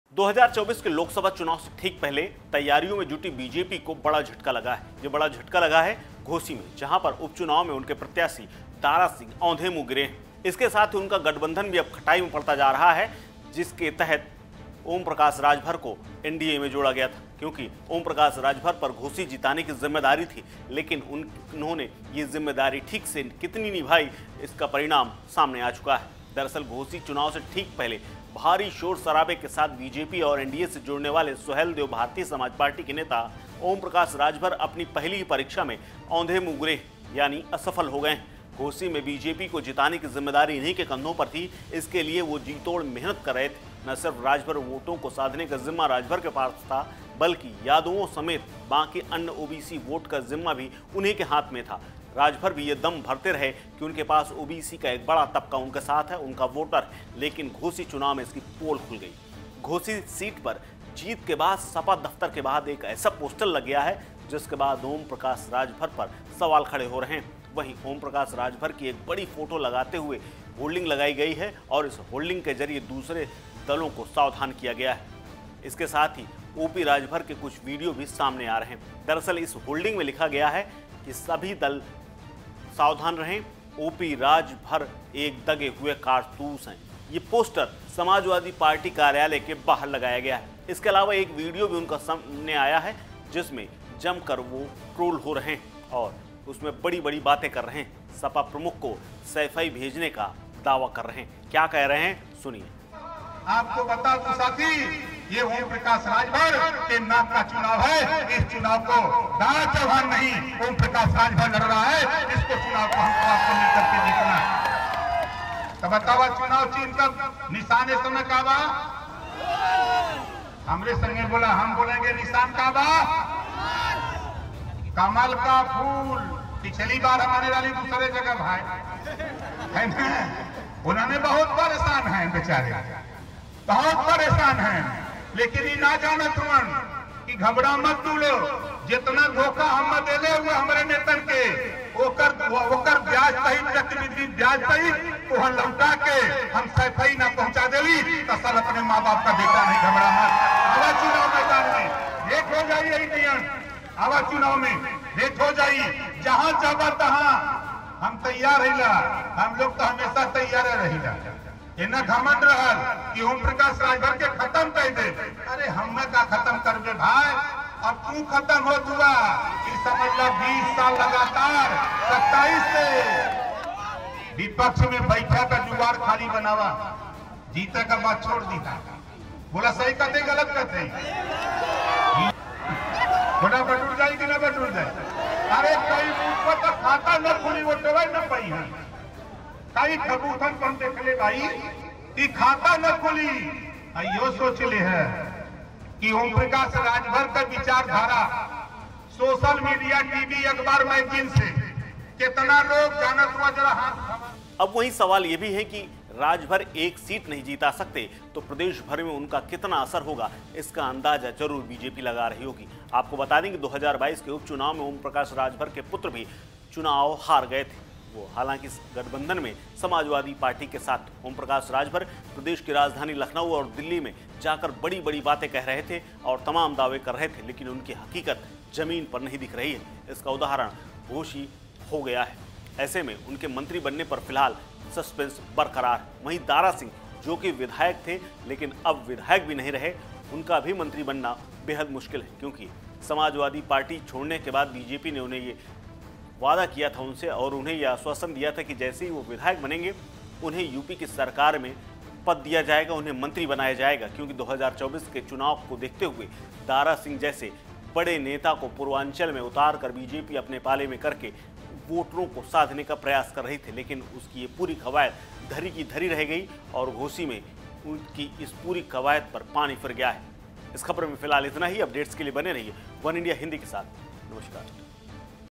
Ghosi में Dara Singh के हारते ही OP Rajbhar के Video पोस्टर हुए वायरल | वनइंडिया हिंदी
घोसी उपचुनाव (Ghosi Poll)में सपा नेता सुधाकर सिंह ( Sudhakar Singh) ने जीत हासिल कर ली है। अब इस हार के बाद सवाल है, कि दारा सिंह ( Dara Singh) की हार की वजह क्या है। वहीं हार के बाद ओपी राजभर (OP Rajbhar) लगातार ट्रोल और वीडियो पोस्टर के जरिए निशाने पर हैं।